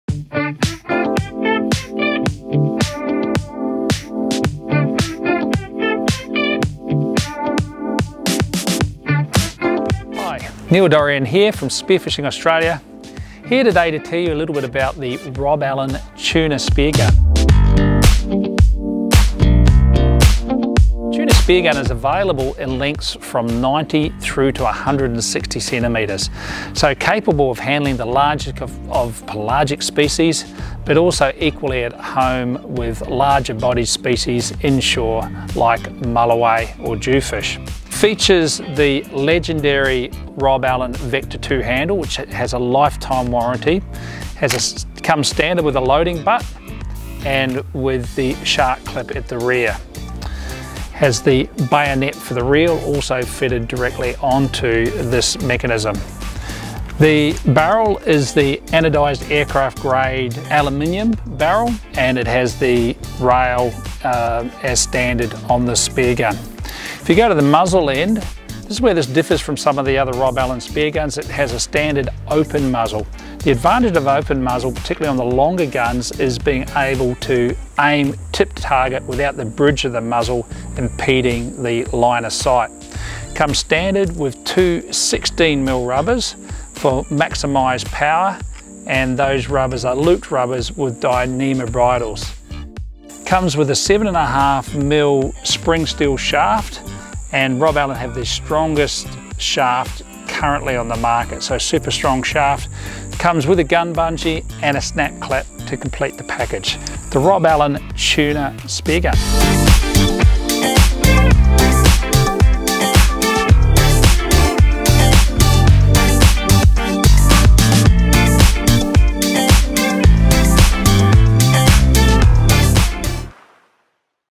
Rob Allen Tuna Speargun
The Tuna’s RA Vecta aluminium rail barrel is manufactured from aircraft grade aluminium with a wall thickness of 1.45mm.
- Next generation Vecta 2 handle and mechanism
- Ergonomically designed loading butt
- Stainless steel line release
- Open muzzle with double 16mm rubbers
- Shaft made from oil quenched carbon alloy spring steel
- High strength, abrasion resistant black mono shooting line
- Reel brackets standard
- Available barrel lengths 1000, 1100, 1200, 1300, 1400, 1500, 1600 mm

More information available at: Spearfishingaustralia.com